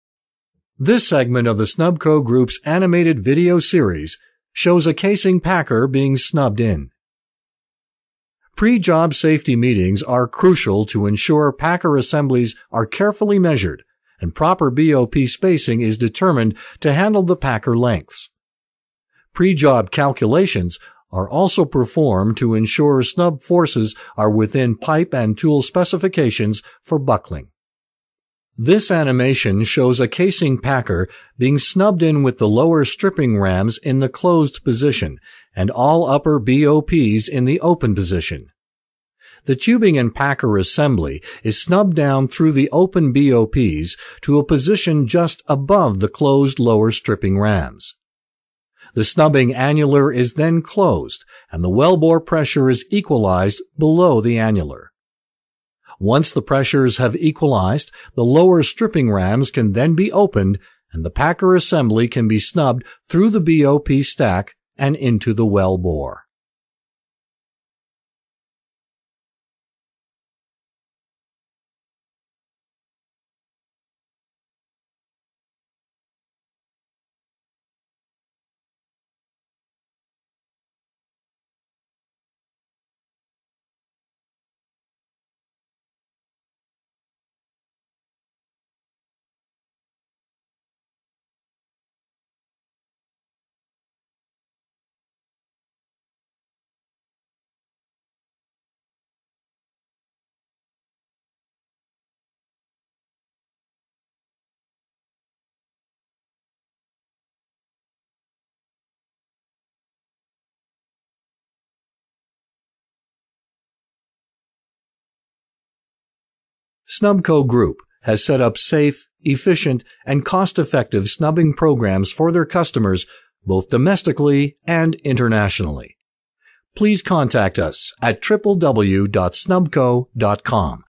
Snubco animated video: Stage in Packer through annular
Snubcos detailed animated educational video demonstrating staging a packer through an annular in a snubbing stack.